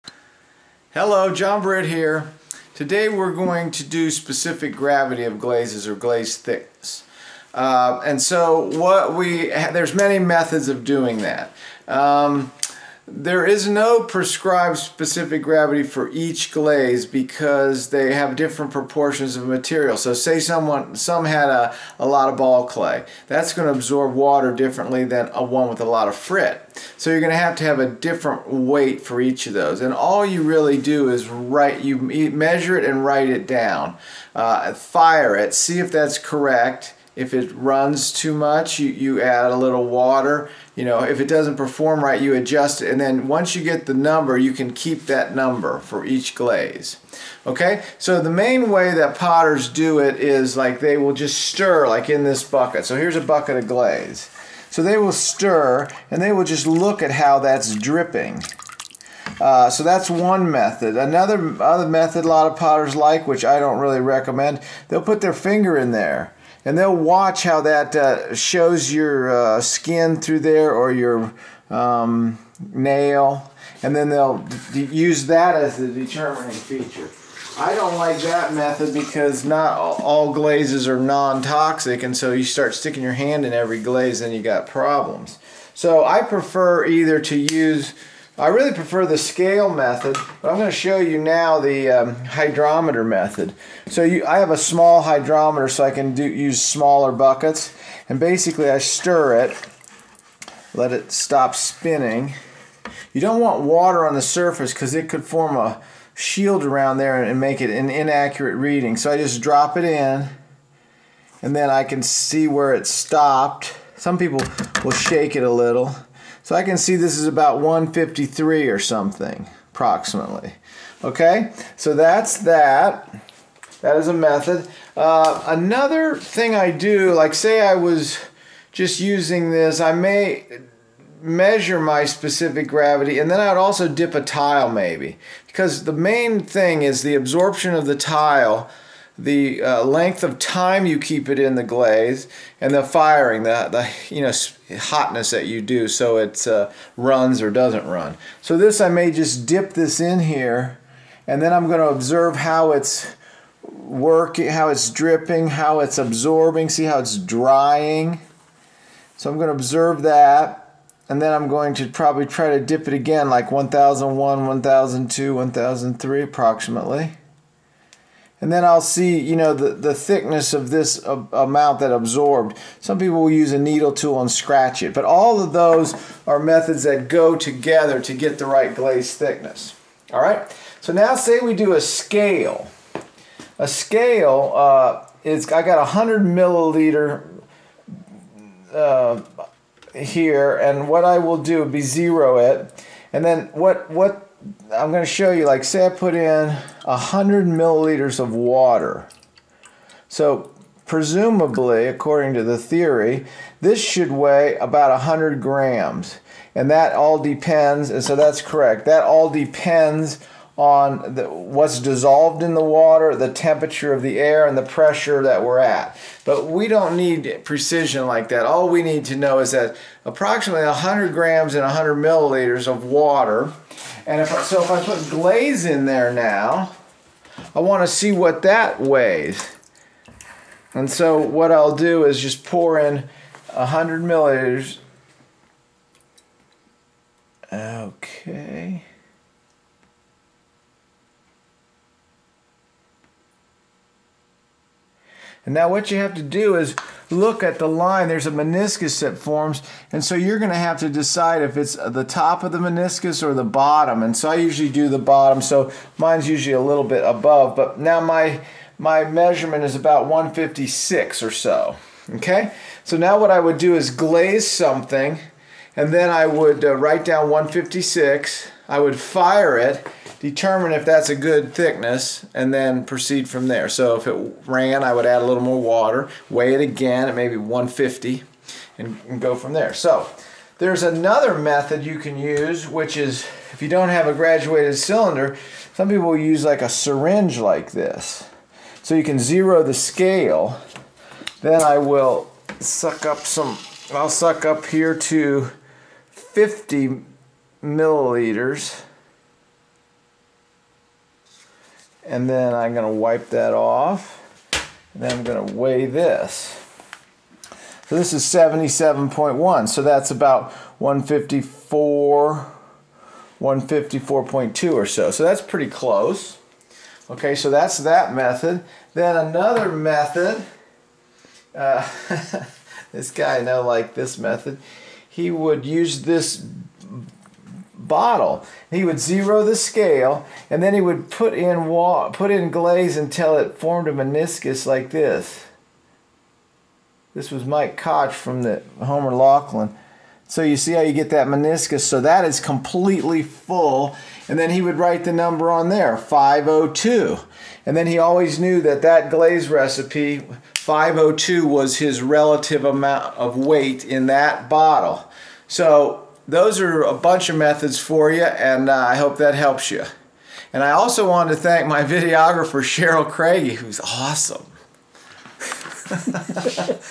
Glaze Thickness or Specific Gravity (Free Online Glaze Course Part 20 Lab)
This is a  short video showing how to measure glaze thickness or specific gravity.